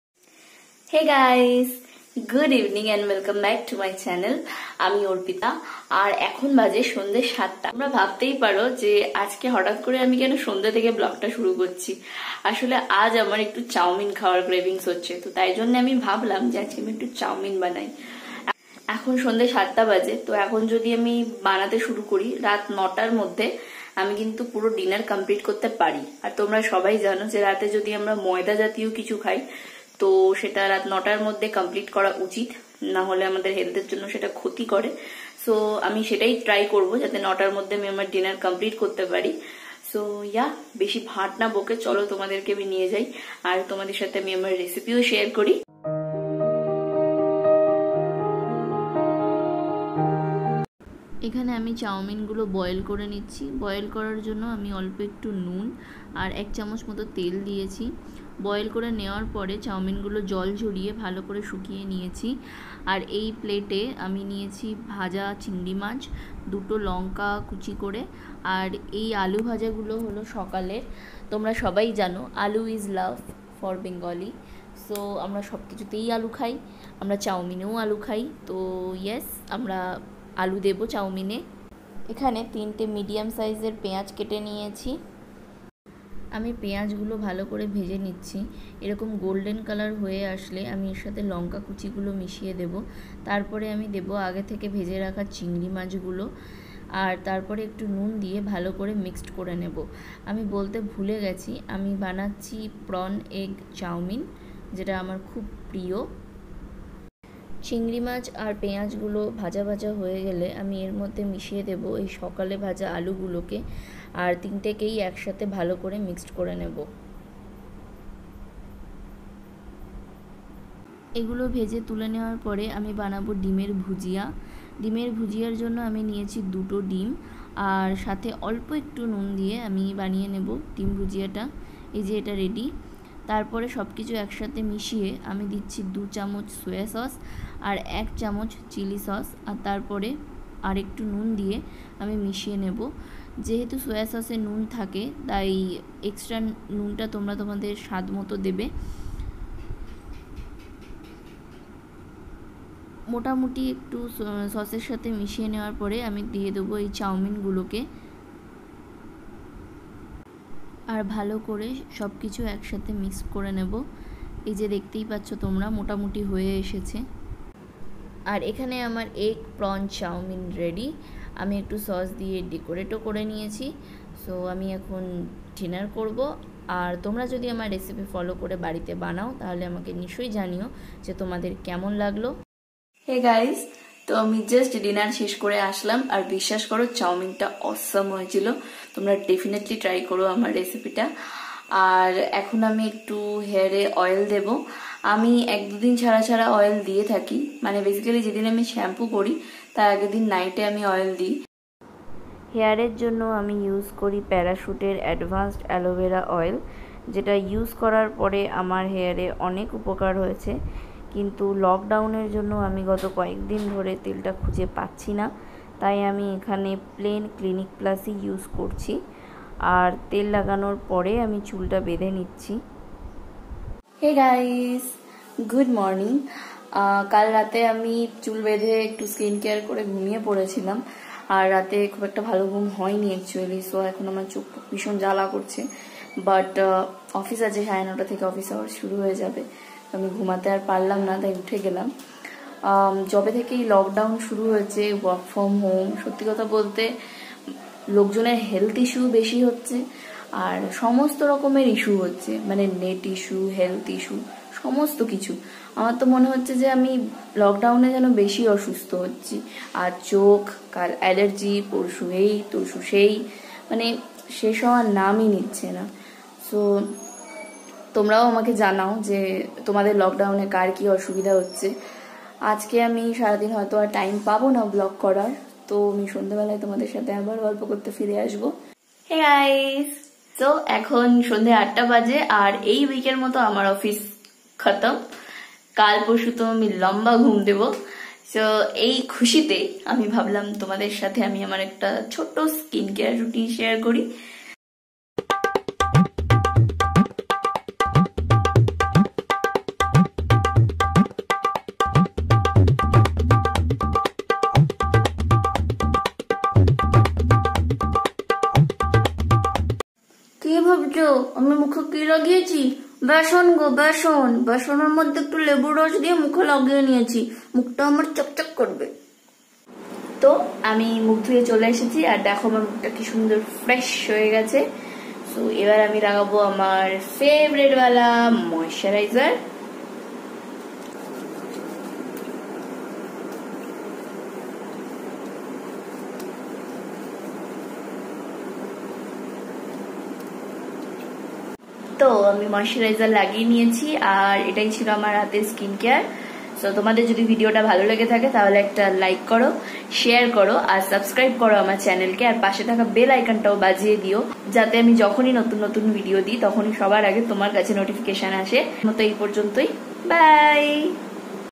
Vlog#7| Mukhe ami aki lagalam? 😝|Prawn Egg Chowmin Recipe
Hello Everyone,

Ami share korechi Prawn Egg Chowmin recipe ar share korechi amr Night skin care routine.

Wanna be my friends??